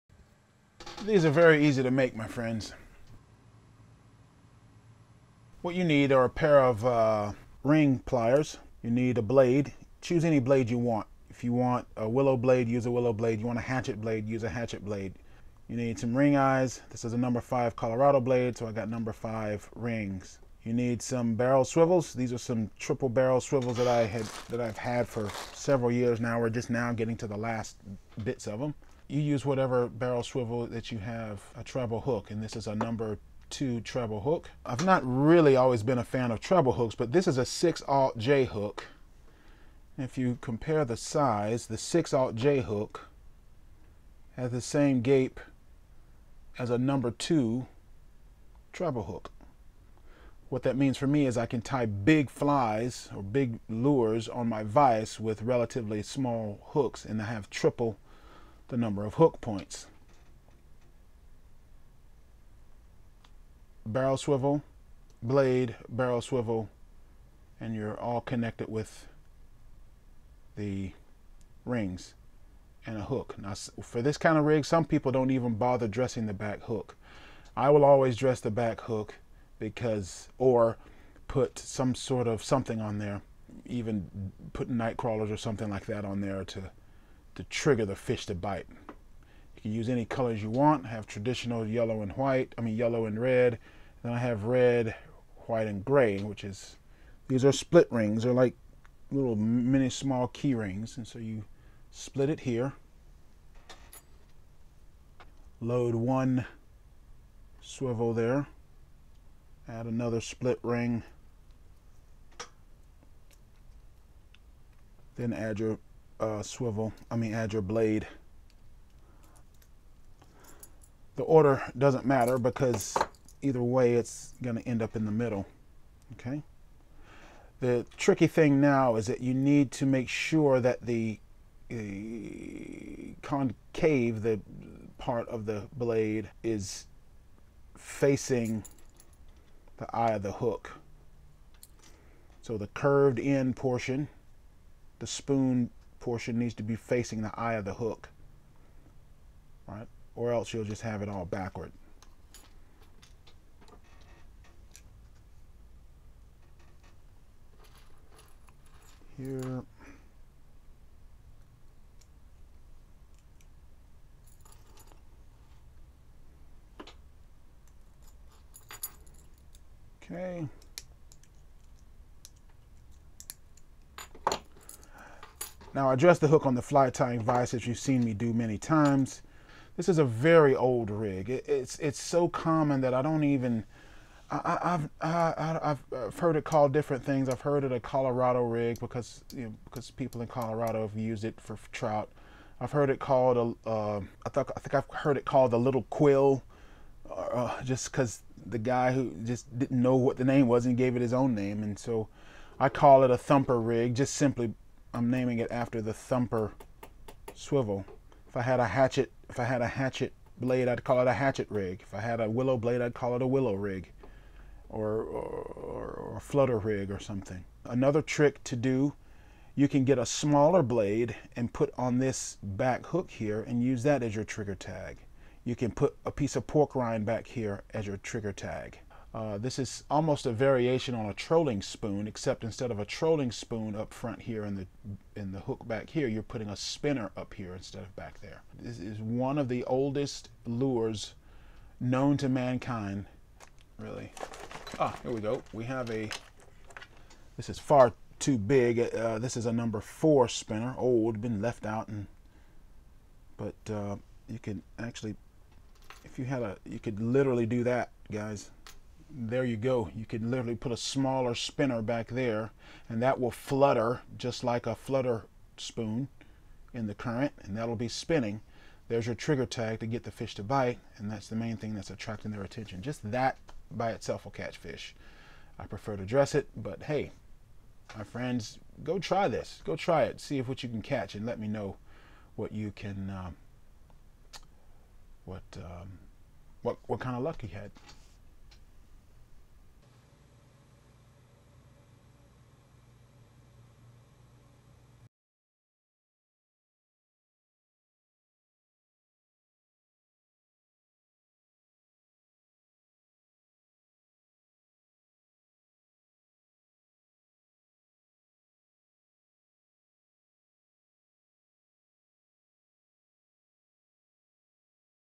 Cheapest Lure Ever Made!
Cheapest lure ever made is what I call the Thumper Rig. I've heard it called a Colorado Rig or a Lil Quill. It is one of the oldest lures. Too bad it has fallen out of favor. It's dirt cheap to make the cheapest lure ever made ever. All you need is a pair of barrel swivels, ring eyes, spinner blade of your choice and a treble hook.

The cheapest lure ever made for fishing often involves using gear you already have. Low cost lures are not low in quality, but simply out of the mainstream. These are oft forgotten rigs that will still hammer fish.